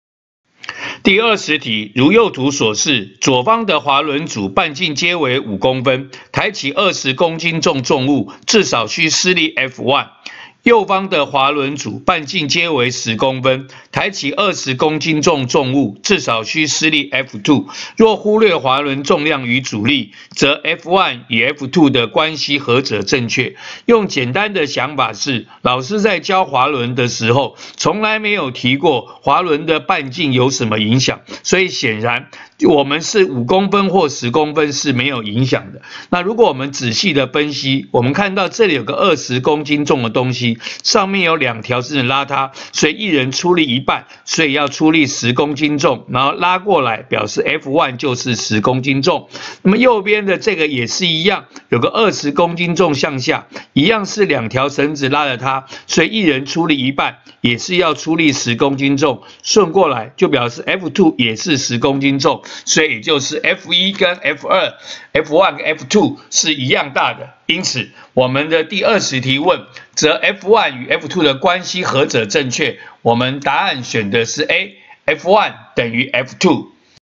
107學年上9年級理化三段第20題
有半徑不同的兩滑輪組，吊相同重物，問兩者施力關係。